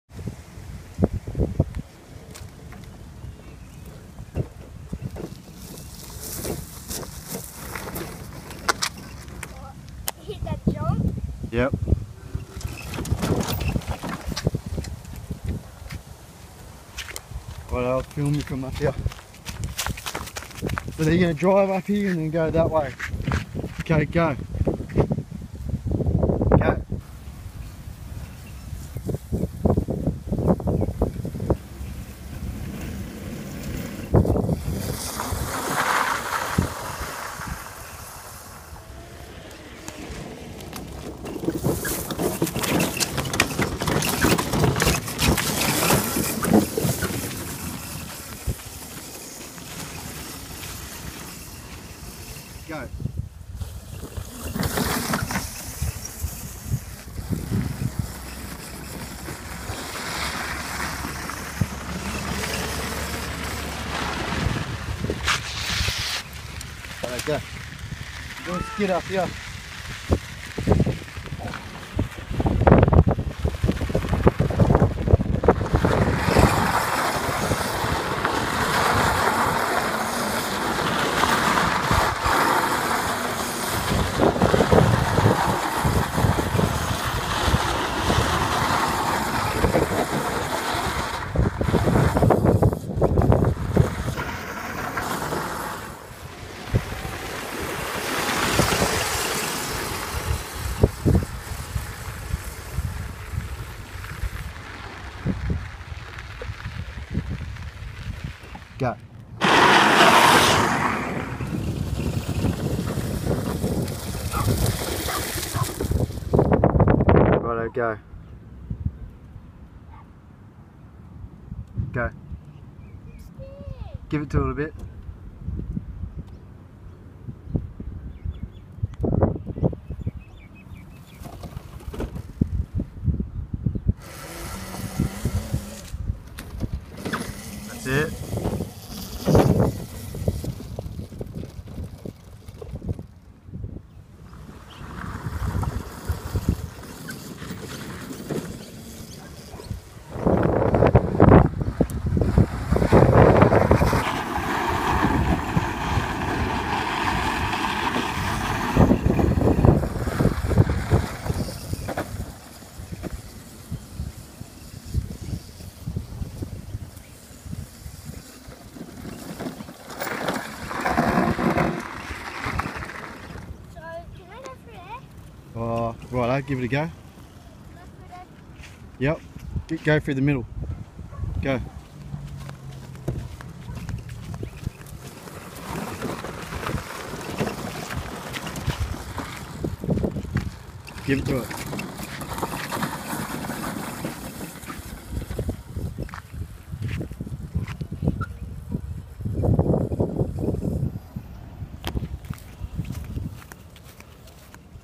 Modified power wheels Lightning McQueen 2017. Best electric ride on cars for kids.
Get your Modified Power Wheels Shirts here.

Full suspension,running a 48v 1000w motor on 36v with a motion dynamics 50amp speed controller.

Our fleet of Modified rides include.

36v Lightning McQueen
36v Mustang
Razor mini chopper
Razor MX350
Razor Crazy cart

Retired cars,with videos include

24v Ferrari
24v Lamborghini
36v Full suspension Lightning McQueen